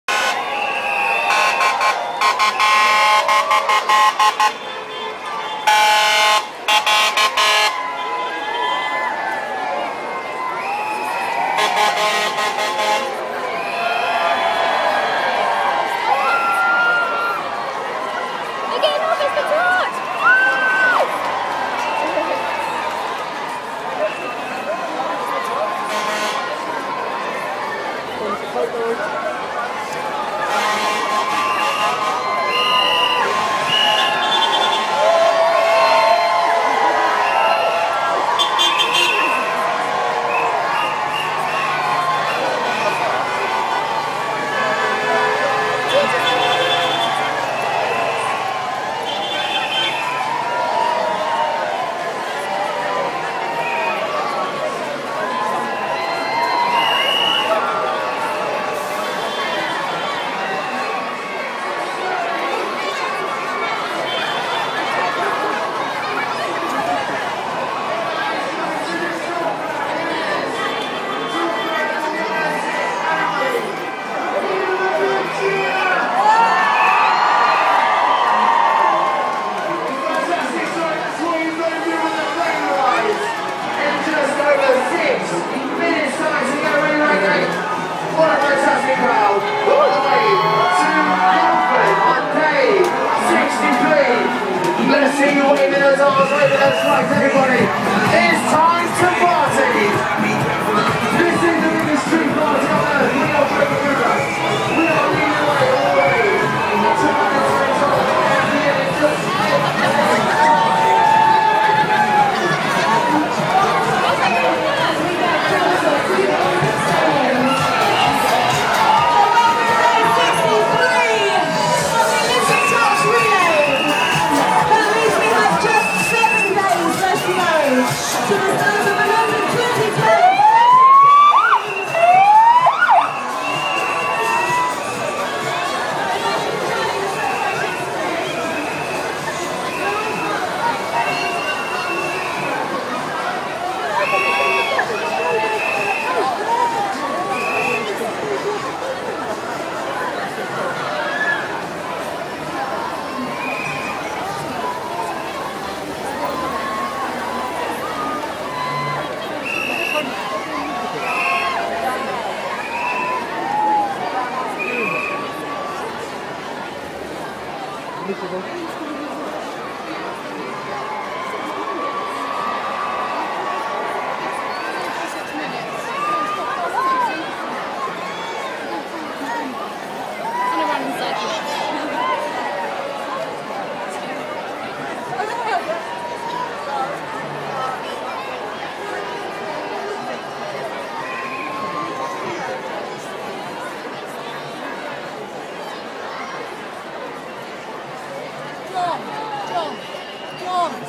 Pre Olympic Torch Procession in Reigate
The build up starts with the sponsors and freebies as it passes under my Space & Time Media offices